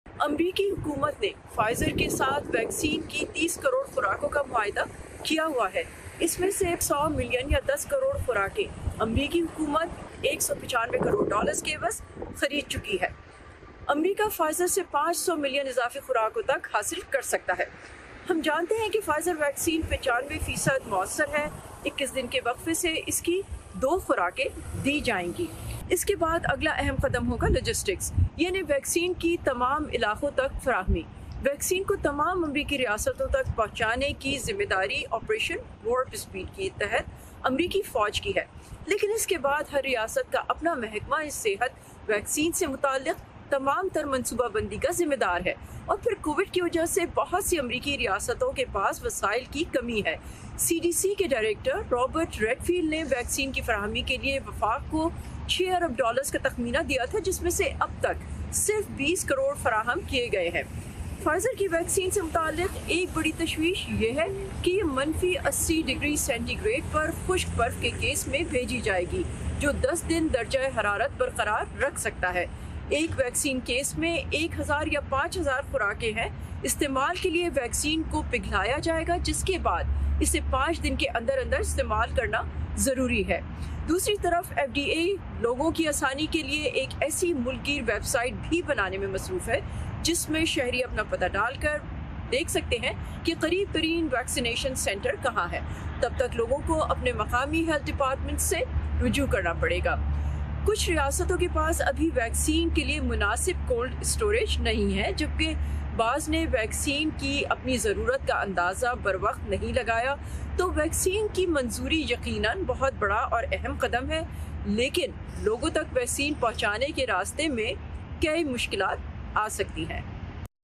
امریکہ: کرونا ویکسین سب شہریوں تک کیسے پہنچے گی؟
امریکہ میں کرونا وائرس کا پھیلاؤ روکنے کے لیے ویکسین لگانے کا عمل شروع ہو گیا ہے۔ فائزر اور بائیو این ٹیک کی تیار کردہ ویکسین پہلے مرحلے میں بزرگ افراد اور طبی عملے کو دی جائے گی۔ مگر کرونا وائرس کی ویکسین عام شہری تک پہنچانے کی راہ میں کئی مشکلات حائل ہیں۔ تفصیلات بتا رہی ہیں صبا شاہ خان۔